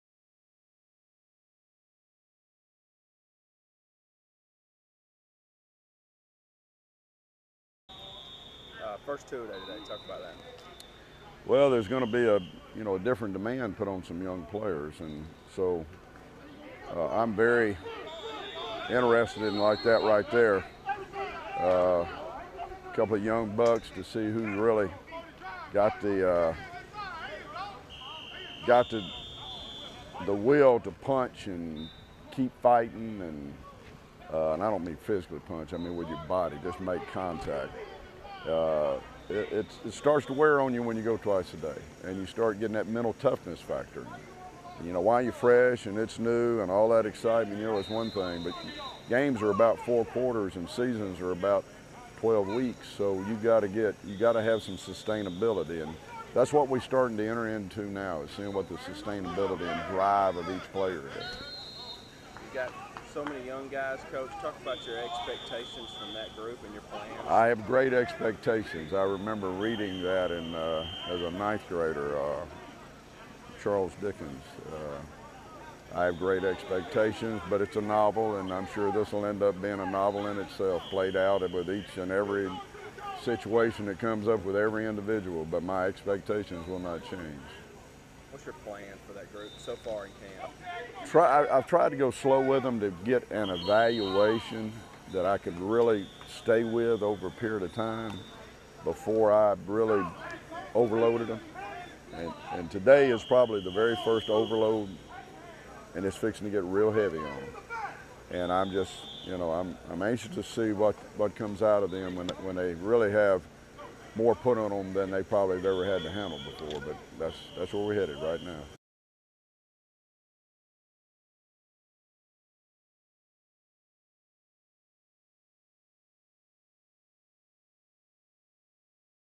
JSU Football Practice (08/14/2012)
Jacksonville State head coach Jack Crowe talks about the Gamecocks practice on Tuesday August 14, 2012